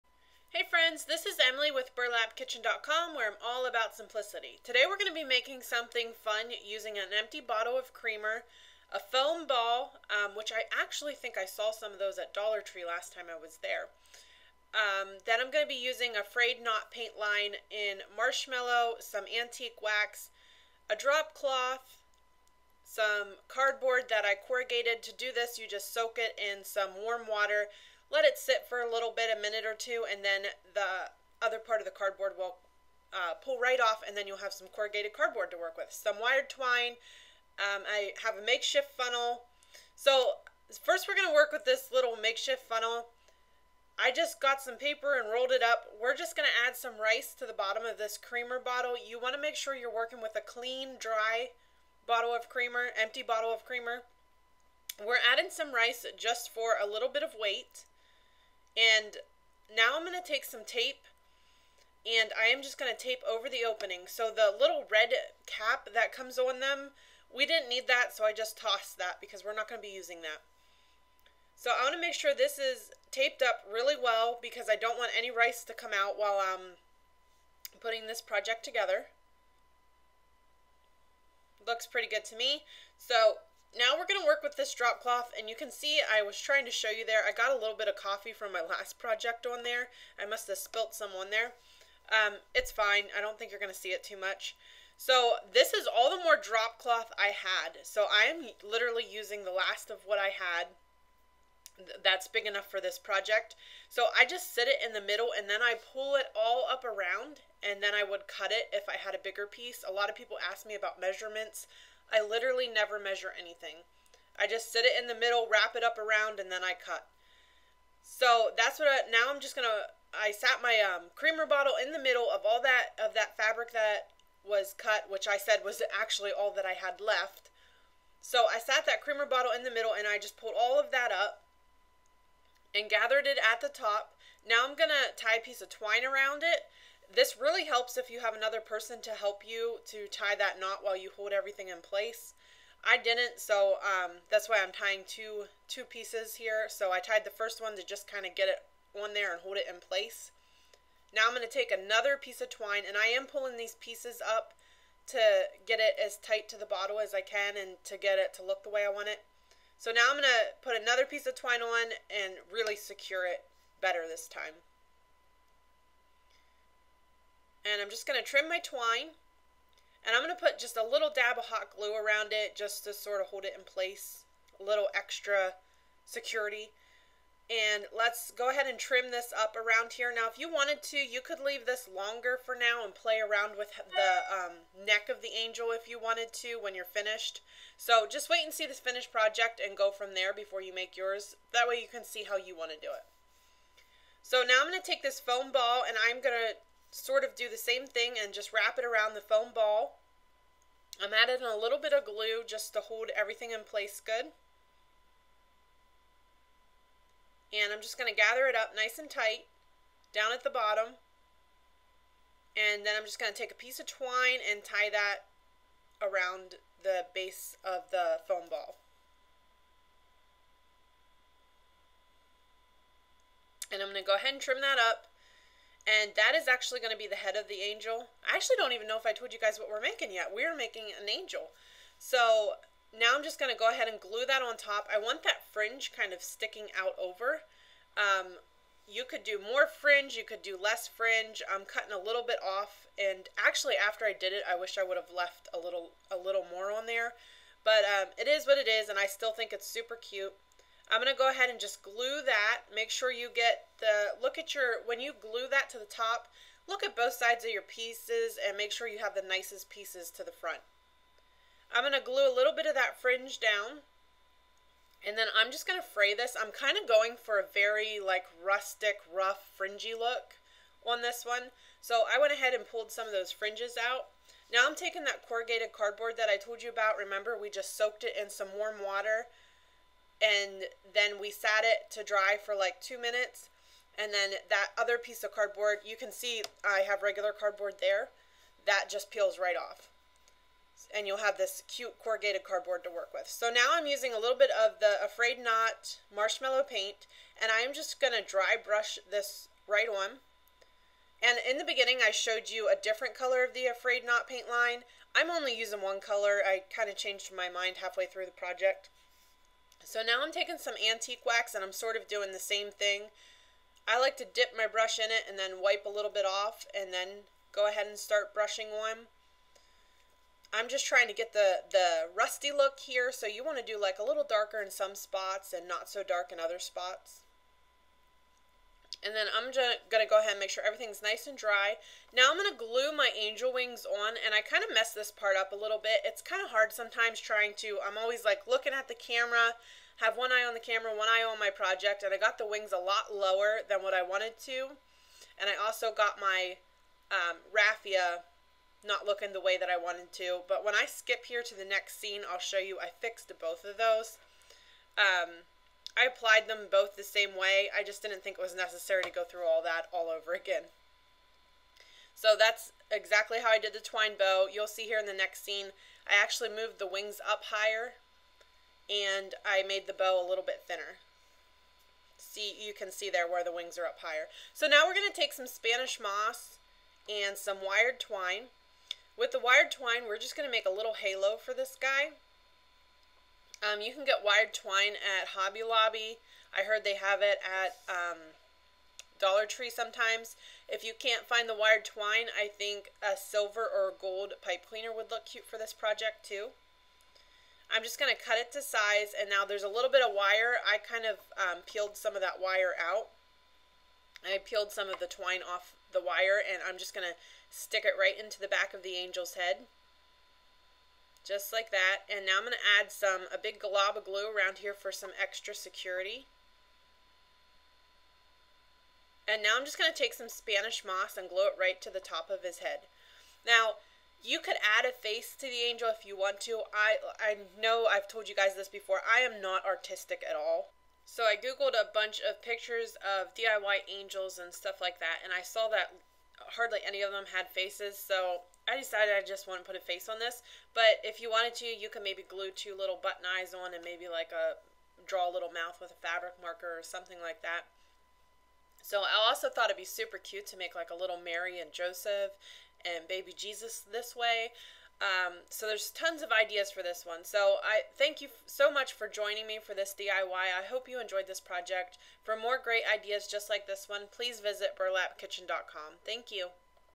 DIY Creamer bottle Angel
This DIY creamer bottle Angel is a great up-cycle project! I think they would make cute Christmas gifts!